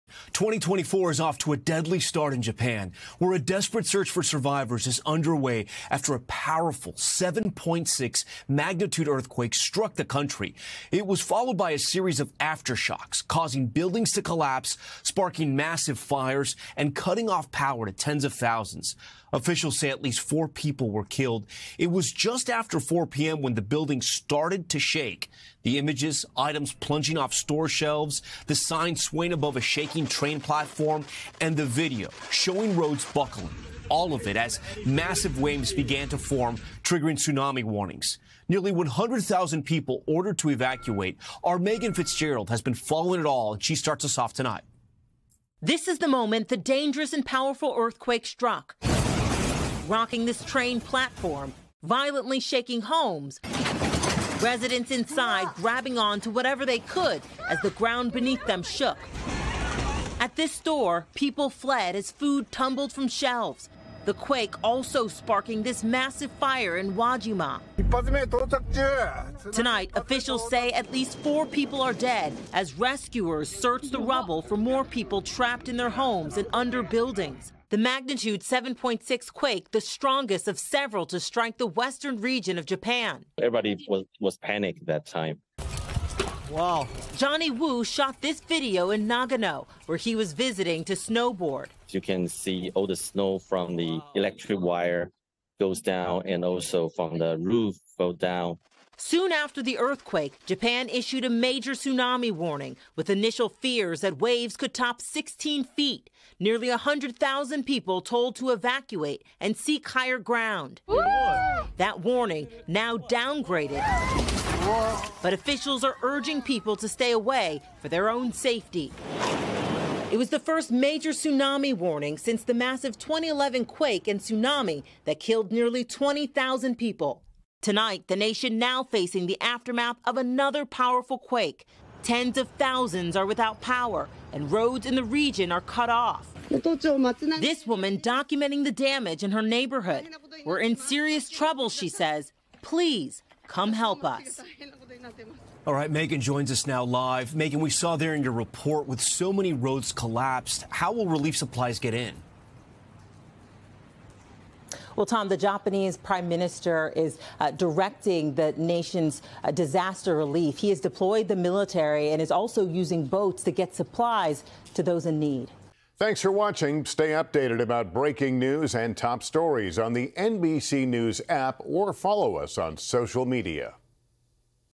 Japan rocked by powerful 7.6 magnitude earthquake
After a powerful 7.6 magnitude earthquake hit Japan, officials say at least four people are dead, with rescuers searching the rubble for more people trapped in their homes and under buildings. NBC News’ Meagan Fitzgerald reports.